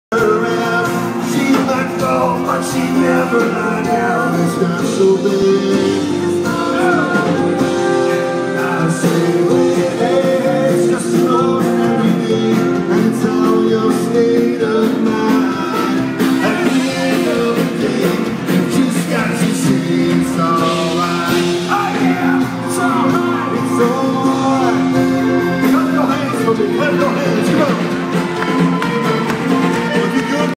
The Great Big Sea
Great time at the Gerald Ford Amplitheatre in Vail Colorado at the Hot Summer Nights tuesday night.   The Great Big Sea played which is an awesome Celtic Rock bank.   Outstanding